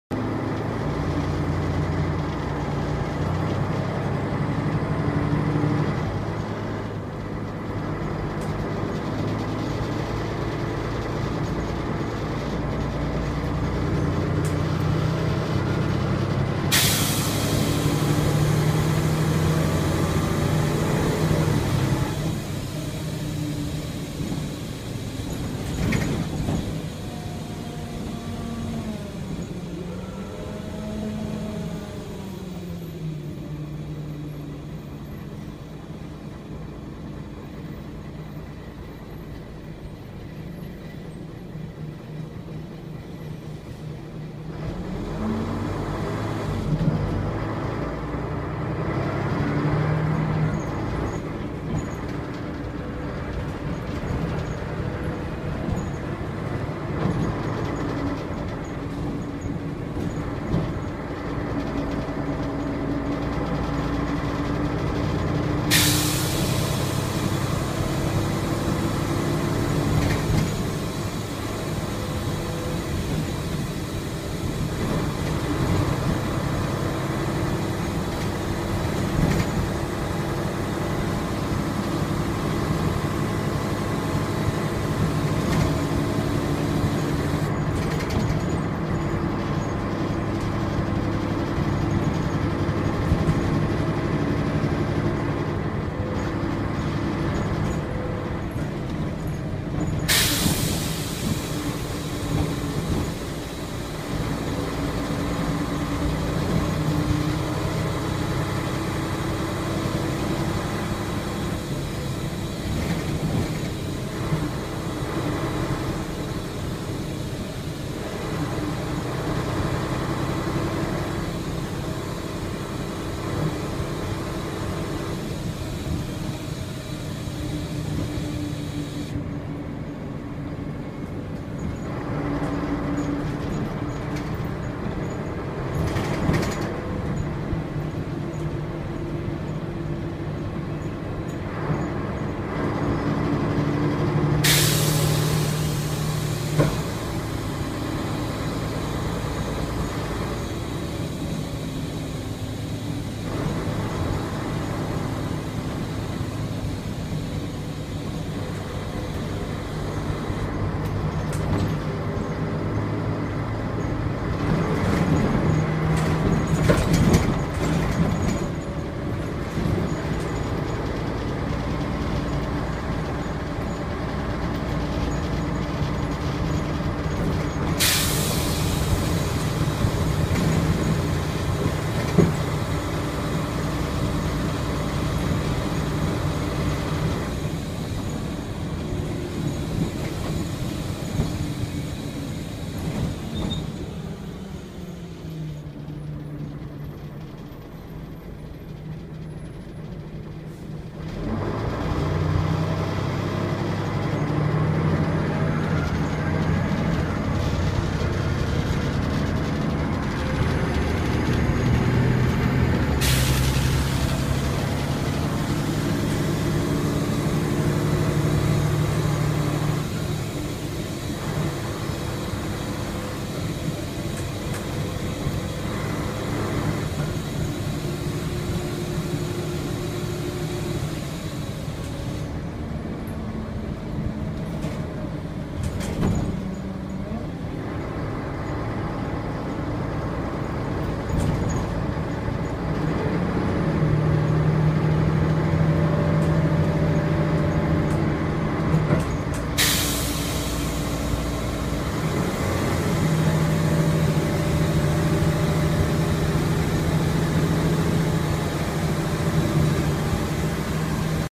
First Kernow 34116 L816CFJ
First Kernow Volvo Olympian/Northern Counties Palatine 34116 L816CFJ is seen on a rail replacement service from Paignton to Newton Abbot.